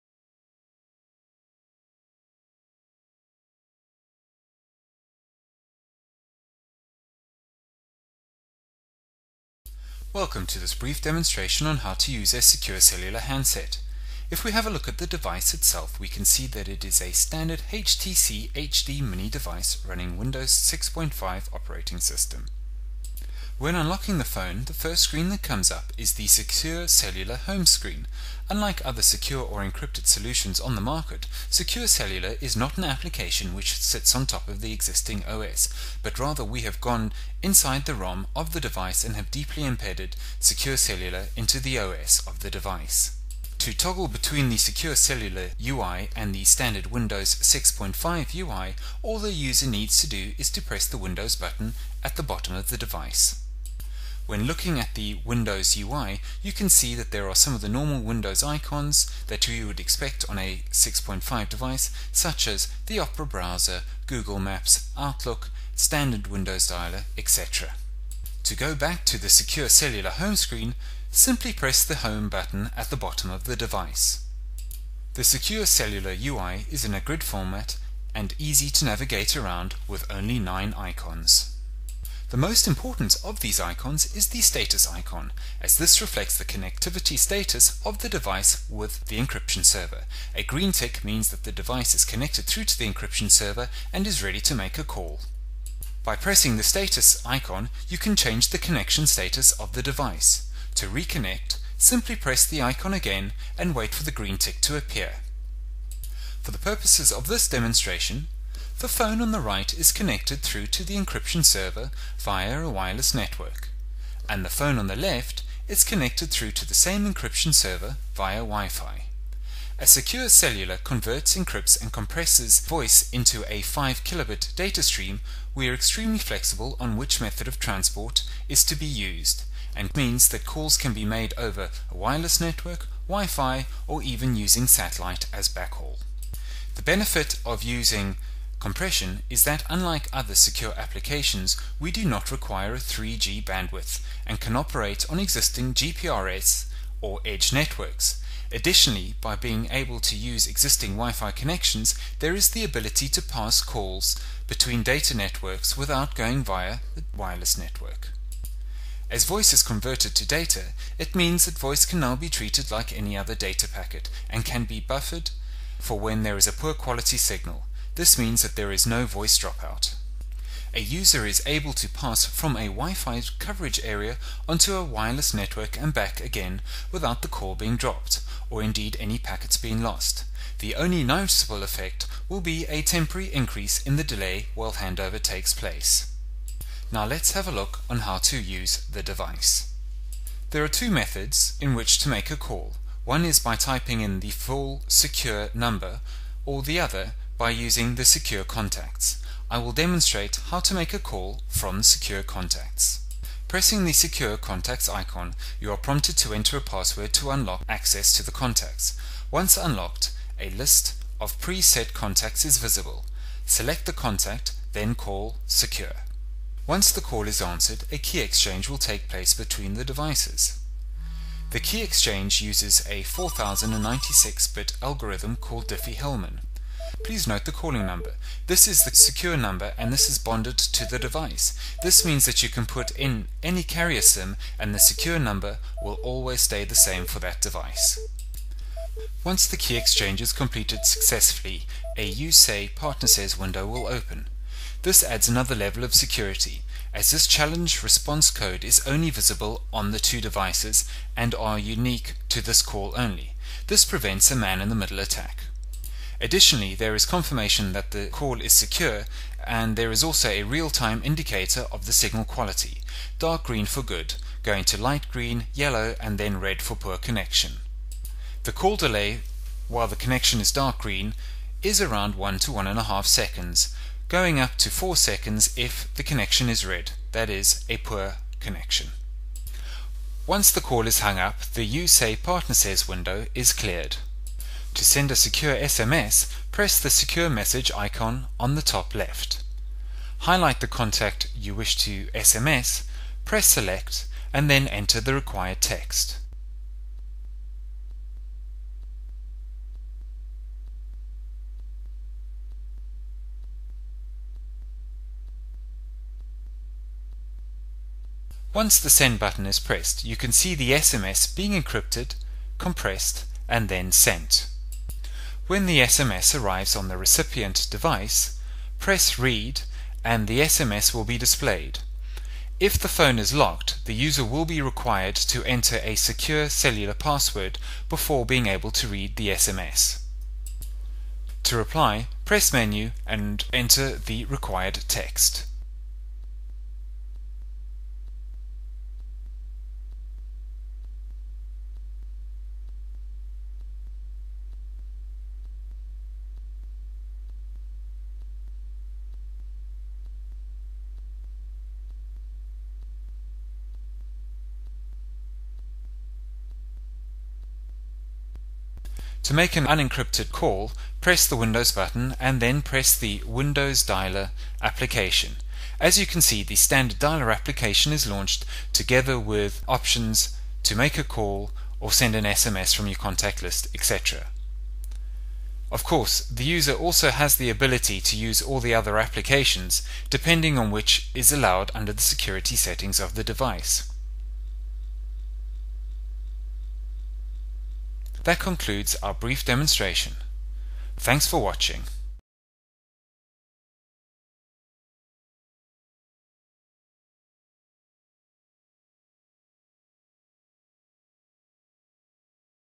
LionHeart Communications - UK Business Secure Cellular - Mobile Encryption - GSM
- Secure Mobile solution that provides interception proof calling via specially developed handset firmware and supporting network. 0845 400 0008